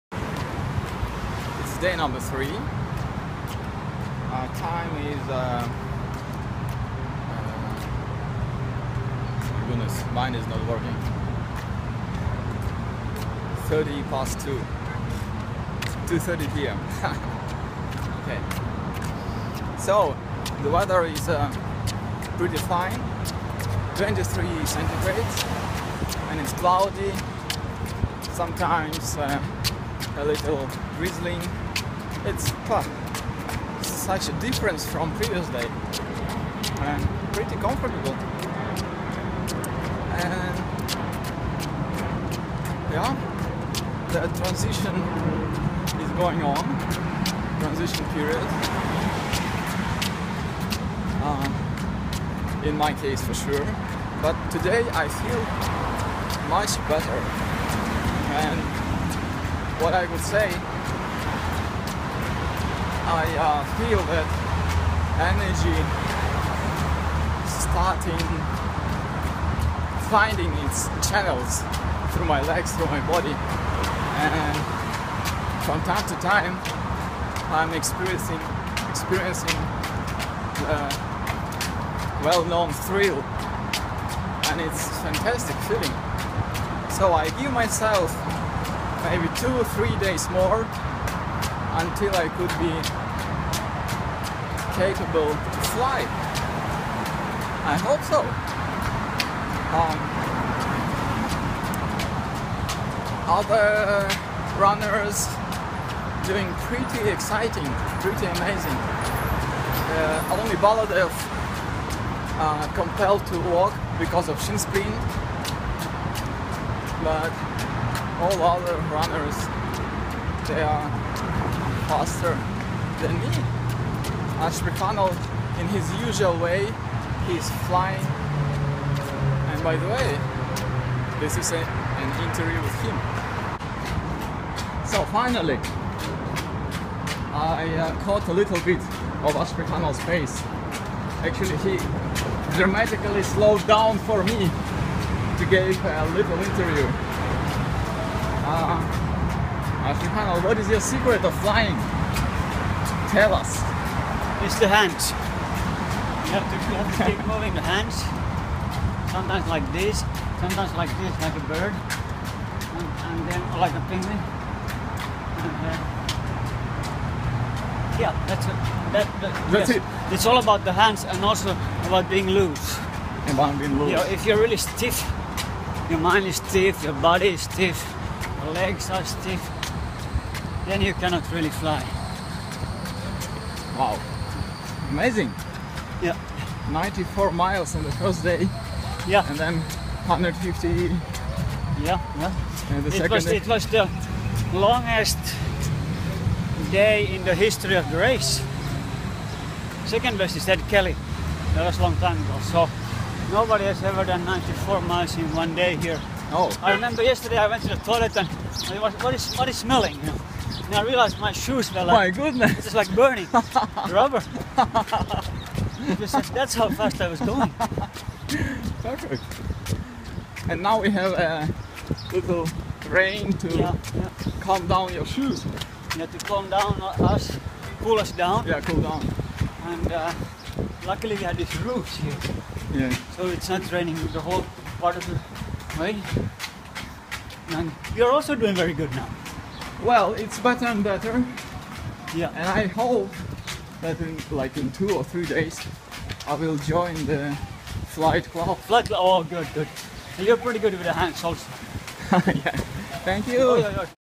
Day 3, 3100 Mile Race 2015
The longest certified 3100 mile race in the world organized by the Sri Chinmoy Marathon Team in New York City, USA.